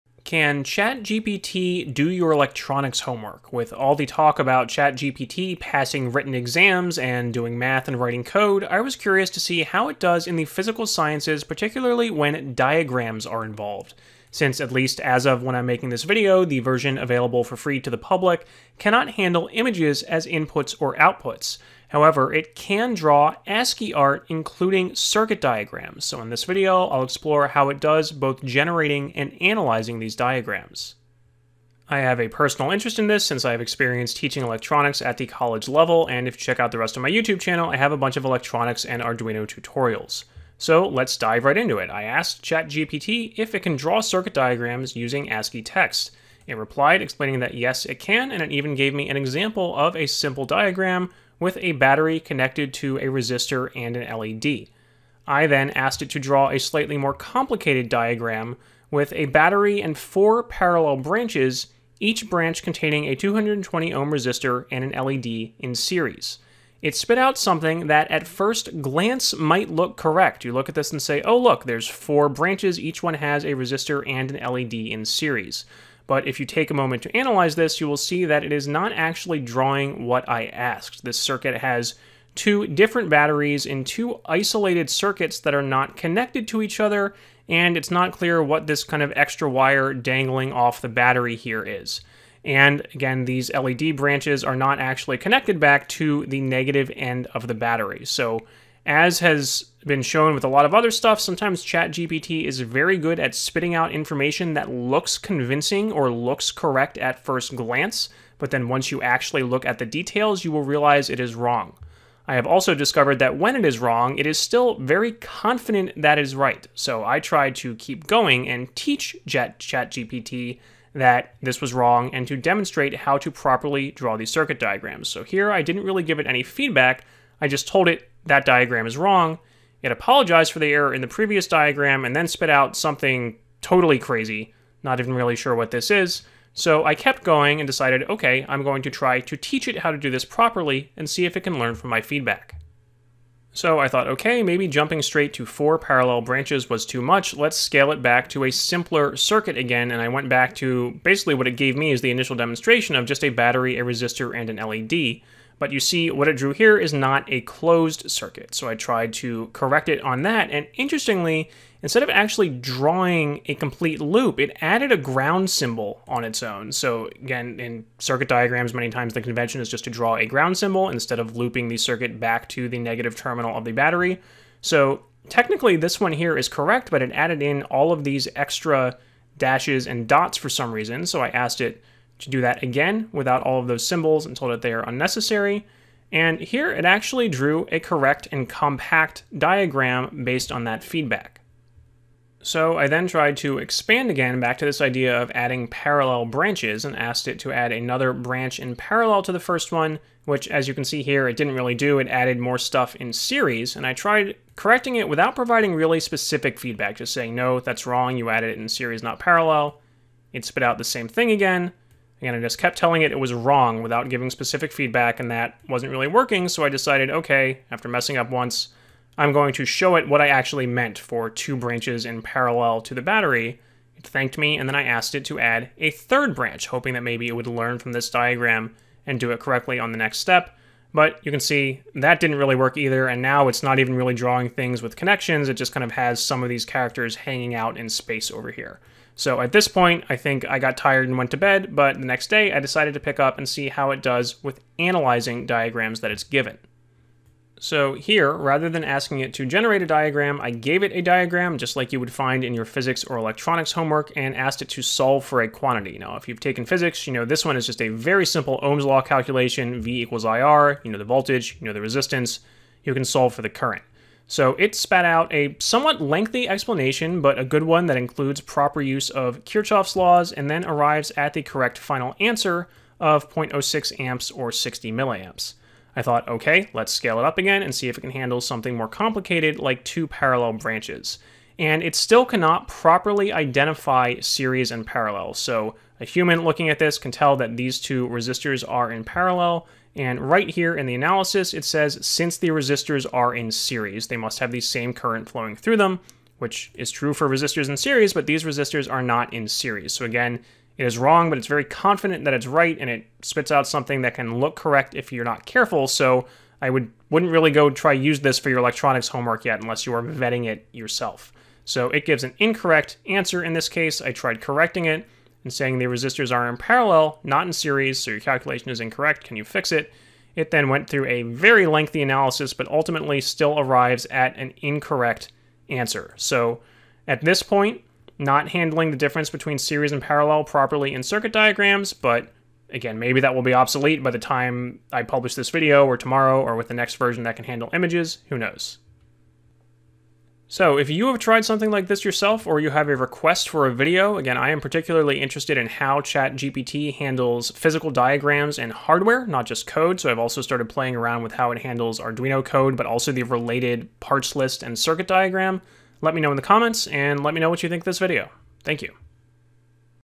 I tried teaching ChatGPT to draw circuit diagrams.
ChatGPT can draw ASCII circuit diagrams, but can it draw them *correctly*? In this video I tried teaching it to properly draw series and parallel circuits. I also checked if it could solve simple circuits like you would see in an introductory physics or electronics class. Have a question or feedback? Leave ac omment!